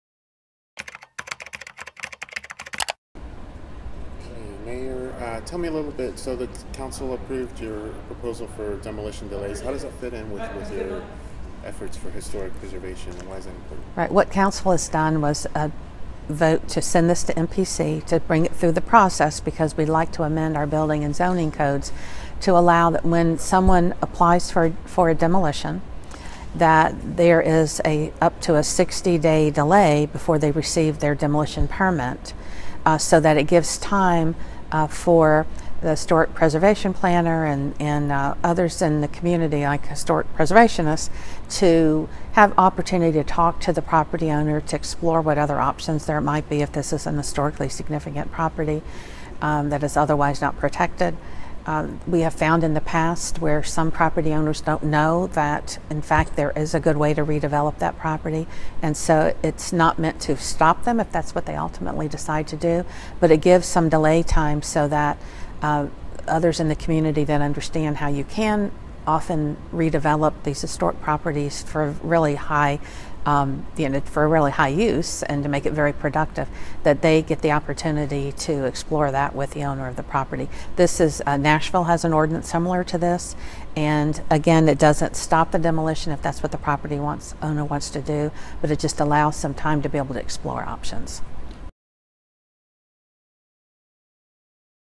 Rogero discusses demolition-delay proposal
Knoxville Mayor Madeline Rogero discusses her proposal, approved by City Council Sept. 2, 2014, that asks the Metropolitan Planning Commission to review proposed permitting delays up to 60 days on houses older than 50 years old. (Video by Tony Hernandez/News Sentinel)